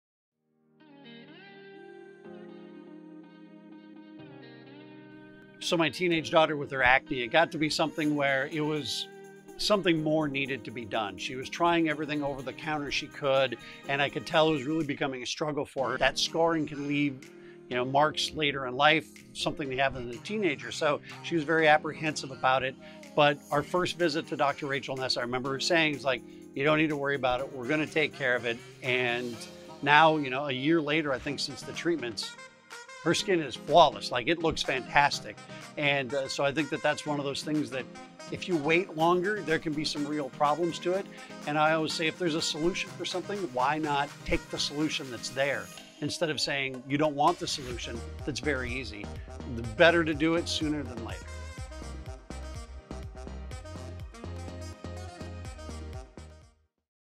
My Daughter's Acne Experience | Fargo Center for Dermatology
Taking care of your child's skin is important and can impact their health for the rest of their lives. Listen to the father of one of our teenage patients explain how Dr. Rachel Ness board certified dermatologist helped his daughter improve her skin. Are you or a loved one in need of dermatology care?